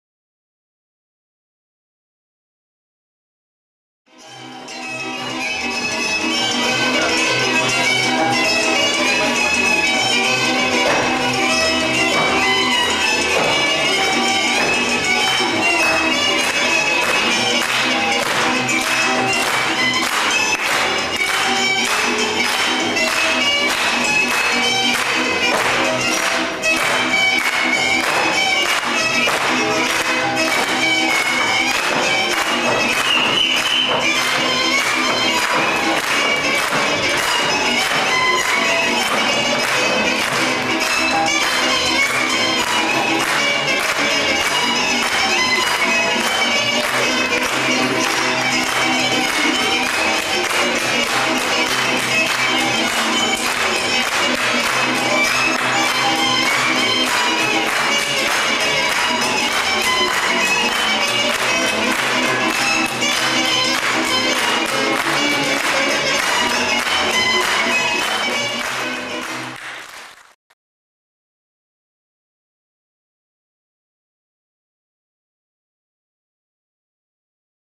Délalföldi frisses
Délalföldi frisses, Ördöngös Néptáncegyüttes, Nézsa, 2007. december 22.